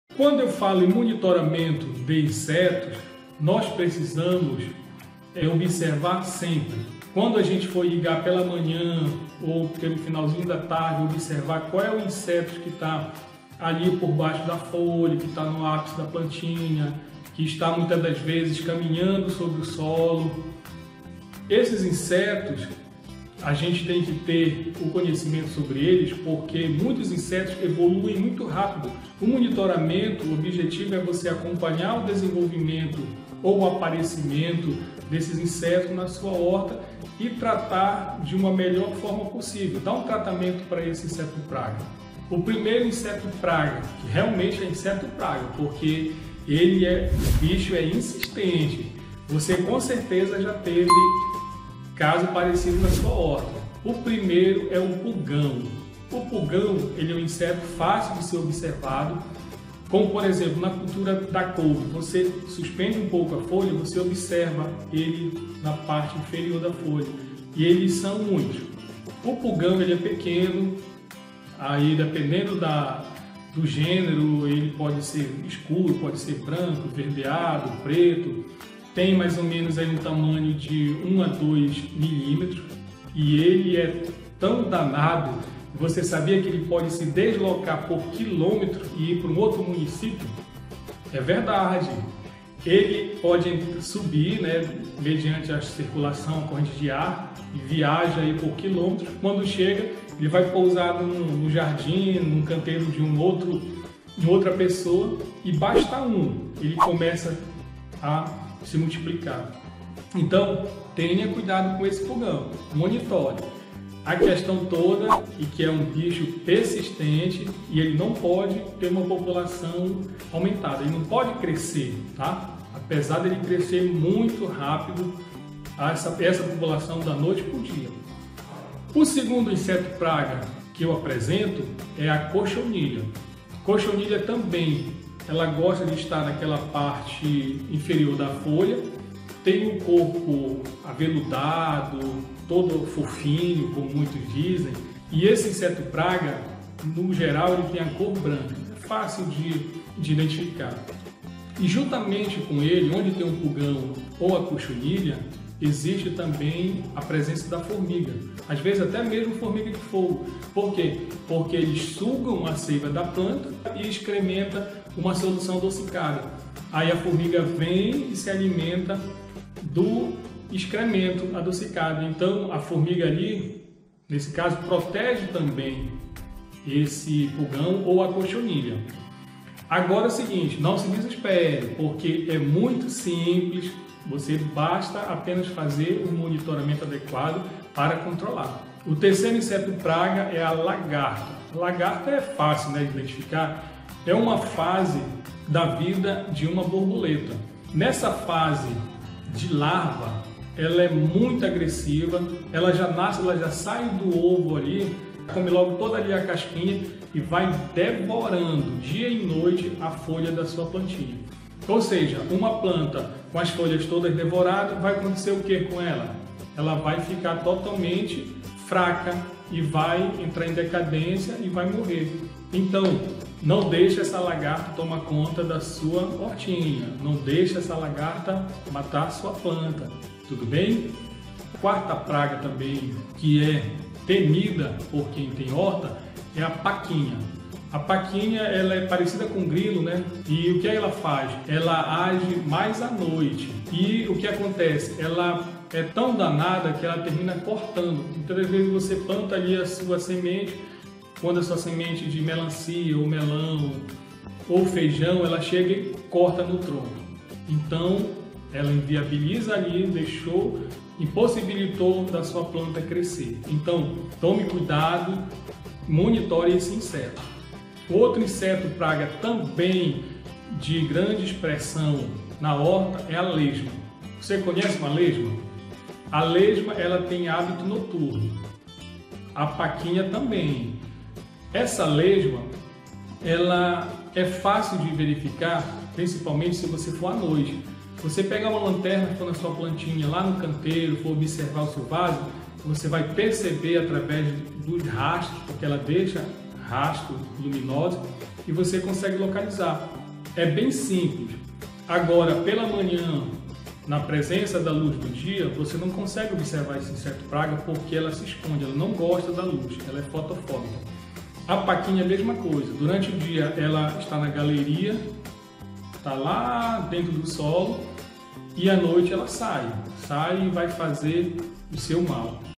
OS 5 PIORES INSETOS DA SUA HORTA
Esses são os cinco piores insetos que podem atacar a sua horta.